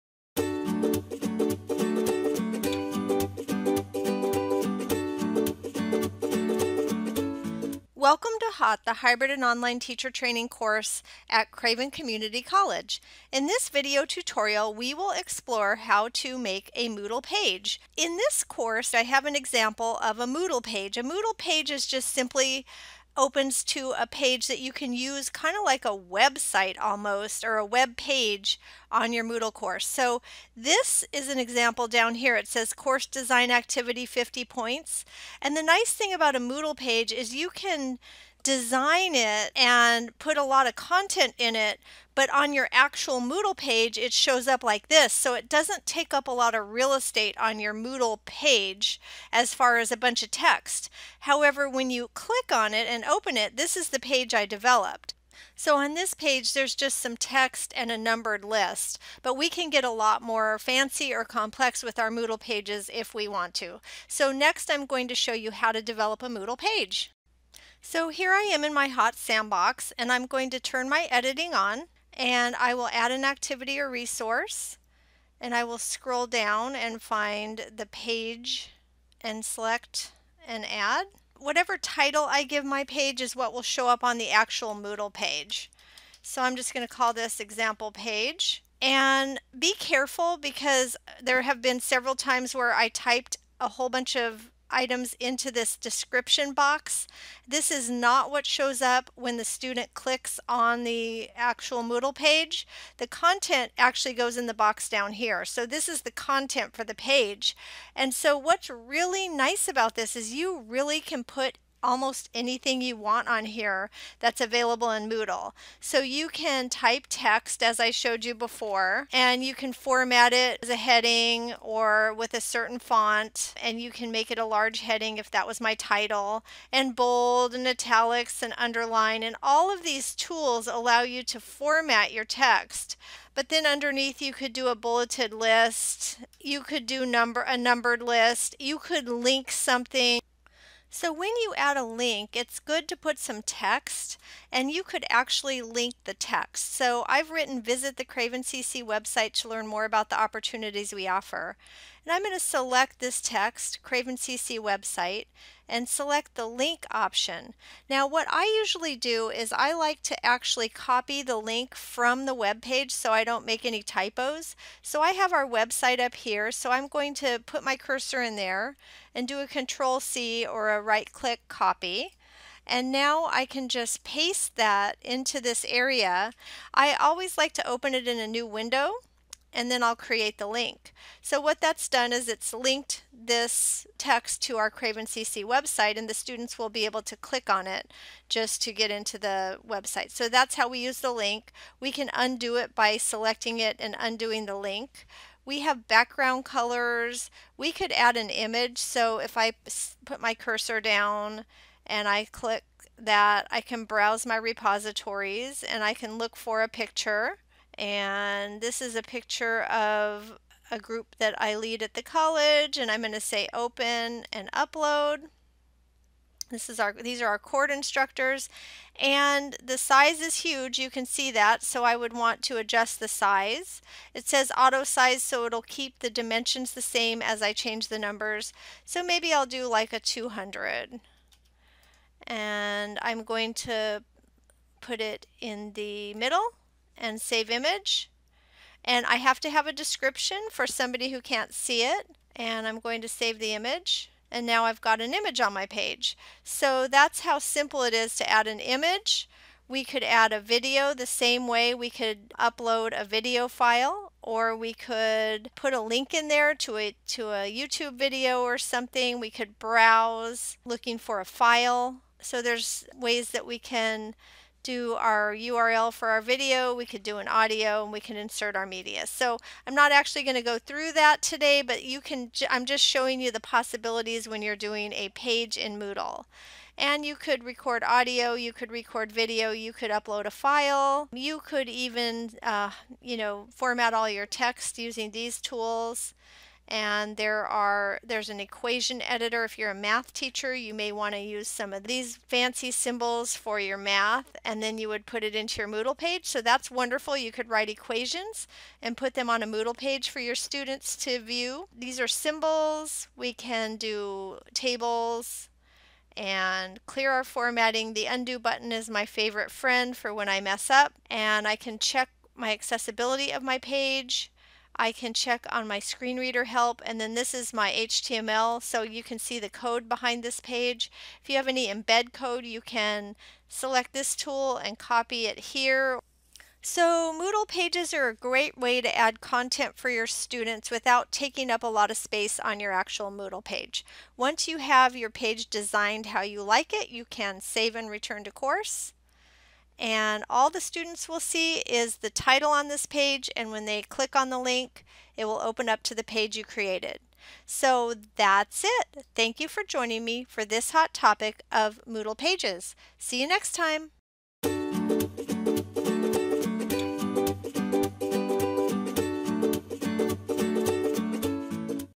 HOTT Moodle Pages
This tutorial demonstrates how to create a Page in Moodle.